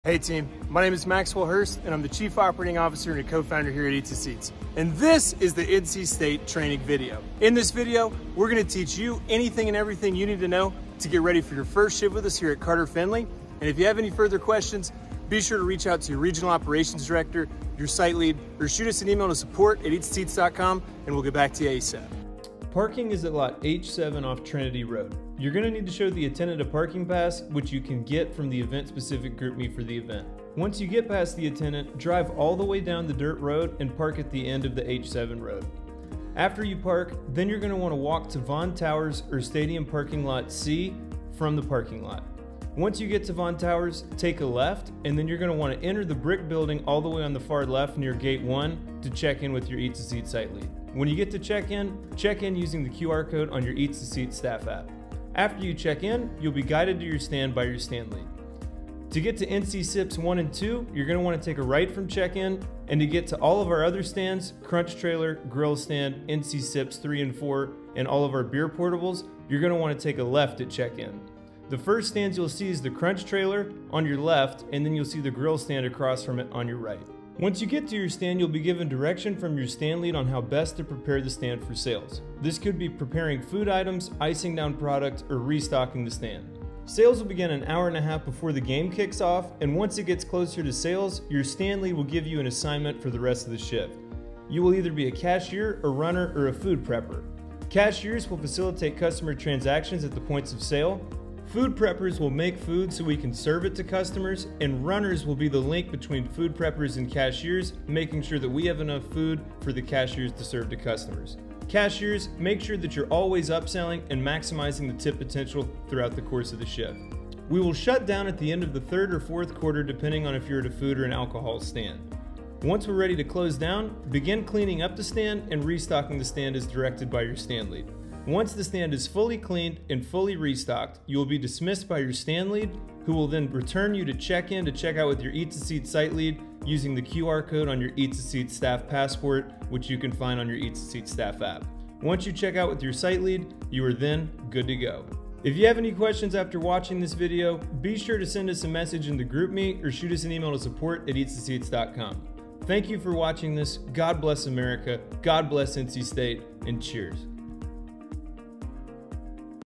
NCSU Football 101
Watch this to get ready for your first shift with us at Carter-Finley Stadium. As a member of the E2S team, you need to know what to expect going into your first shift. Below is more information that can also be found in the video:

How to get signed up:
1. Download the E2S Staff App and complete your onboarding.
- Have to complete all of your paperwork before coming on-site
3. Sign Up for upcoming events on our app
- This is the only way we are going to know you want to work!
4. Join our E2S x NC announcement GroupMe
5. Complete your MANDATORY background check on our app
6. Link to our MANDATORY TEAM Training instructions

General E2S Policies:
1. Dress Code
- All black, black hat, black non-slip shoes, black socks, black belt
2. Tardiness
- 4 unexcused tardies allowed at max
3. Call-outs
- 2 unexcused call-outs allowed at max
4. Written Warnings & Appeals
- You will receive a written warning after each infraction

What's Next?
You can also reach out to your Regional Operations Director, your Site Lead, or anybody with the E2S Admin team for help. Be sure to register for events through the E2S staff app on your phone to sign up for upcoming events!

Contents: